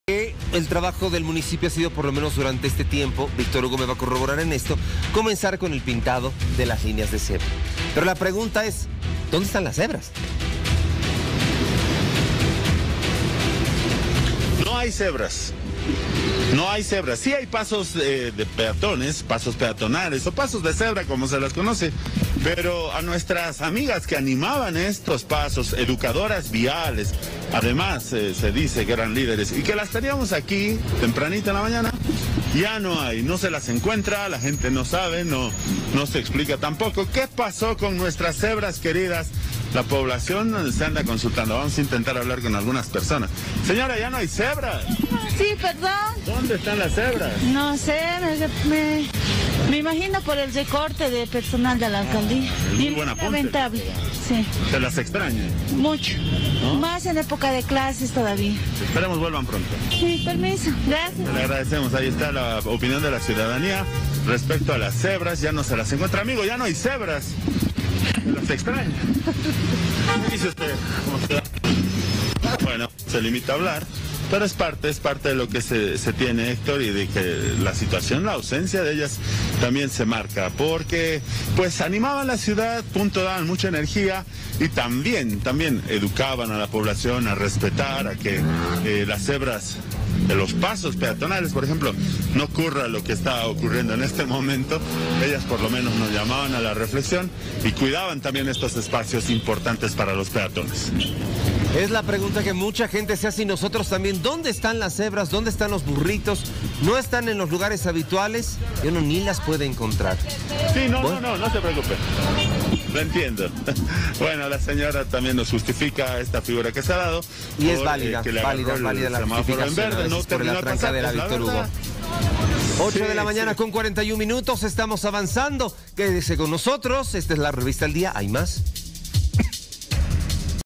¿Dónde están las cebras?
En un completo reporte de La Revista Al Día le mostramos en detalle cómo se realizaron los trabajos de pintados de los pasos peatonales, sin embargo no hemos encontrados a las cebras en las calles paceñas.